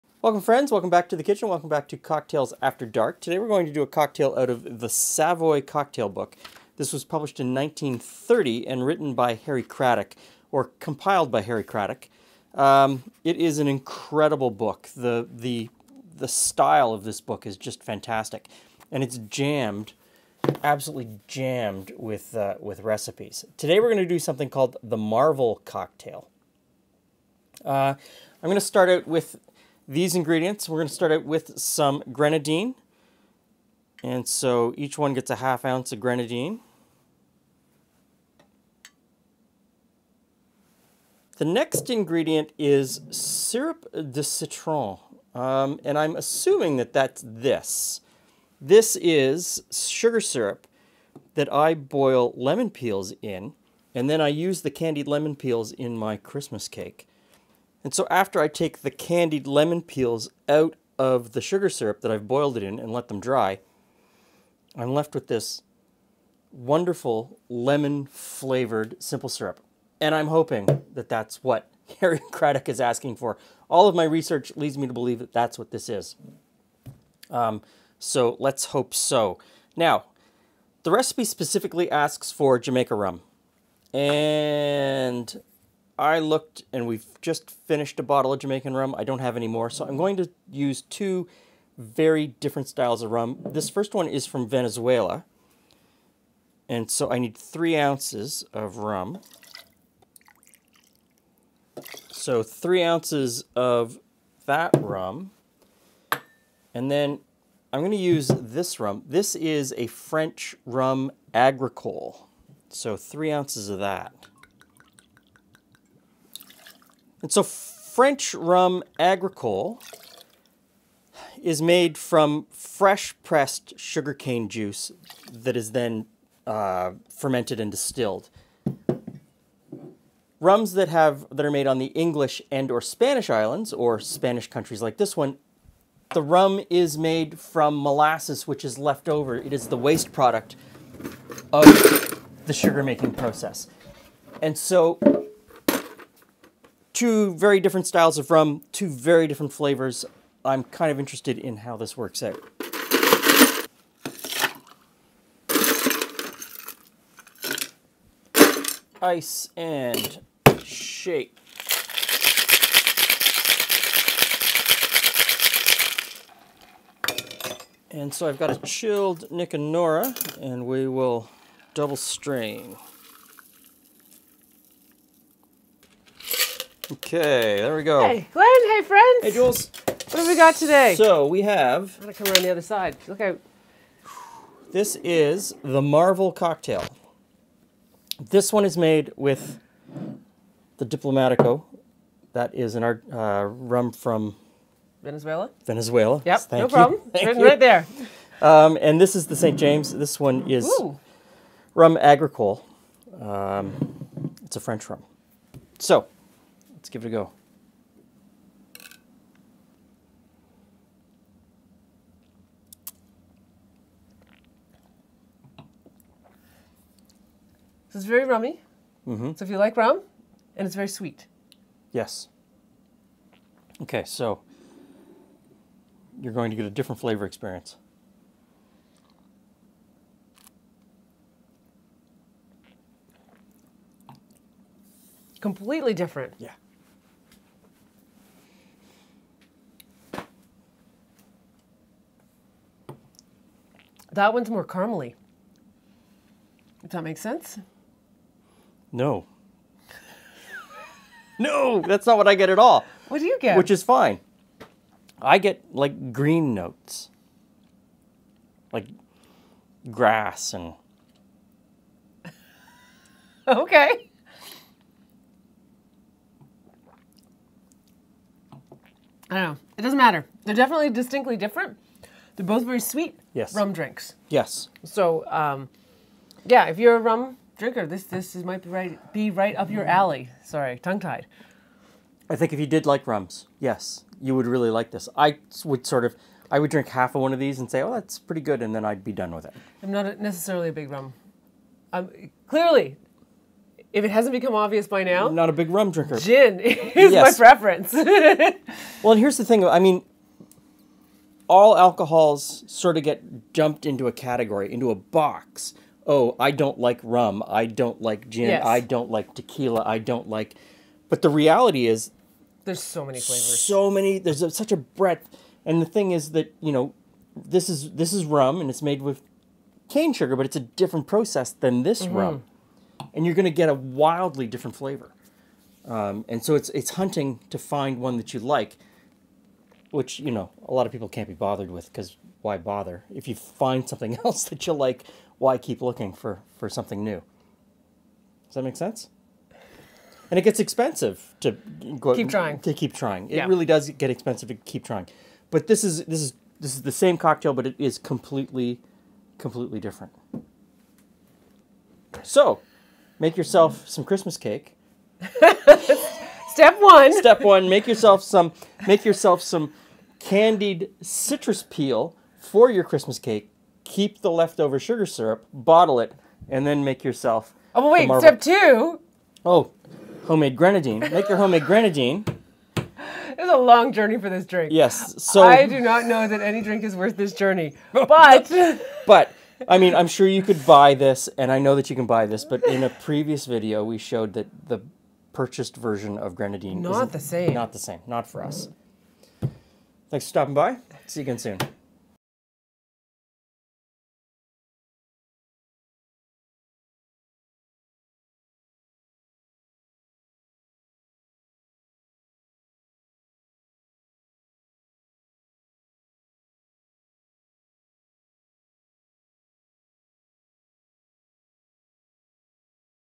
1930 Marvel Cocktail - Cocktails After Dark
1930 Marvel Cocktail - Glen And Friends Cocktails After Dark
Today we make another of the Prohibition Era Cocktails from the 1930 Harry Craddock cocktail book 'The Savoy Cocktail Book'. This is a relatively simple 3 ingredient rum cocktail.

Ingredients:
¾ Jamaica Rum
1/8 Sirop-de-Citron
1/8 Grenadine

Shake well and strain into a cocktail glass